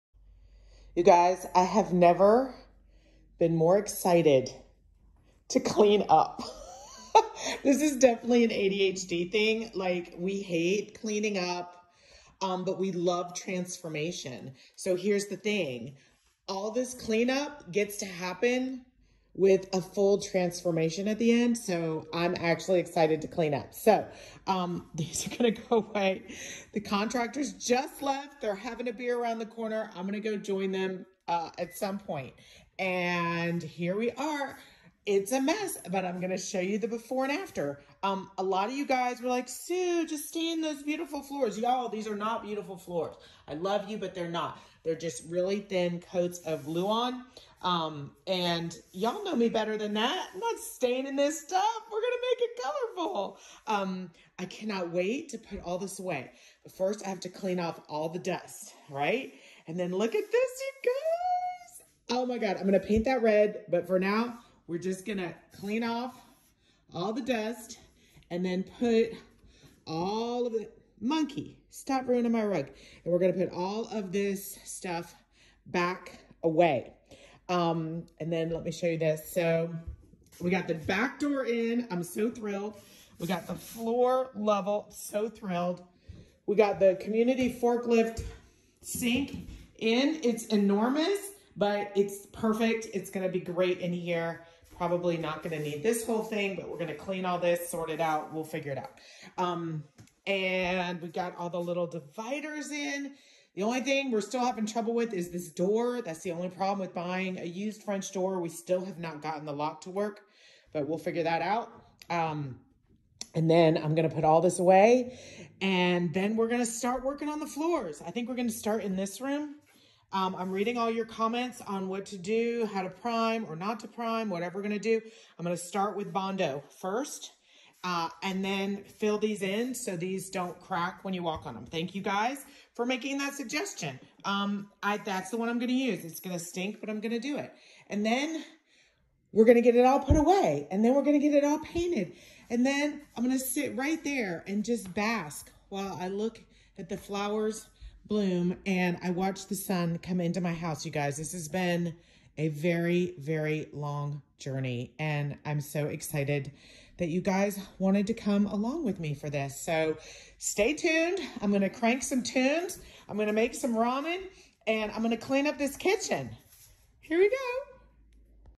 Anybody with adhd knows that cleaning sucks unless you get a big win, a huge transformation or an u
Anybody with adhd knows that cleaning sucks unless you get a big win, a huge transformation or an upcoming party. This will be then only tome im this excited to clean my kitchen 😂. adhdinwomen adhdhacks adhdcleaning wanna join my adhd community?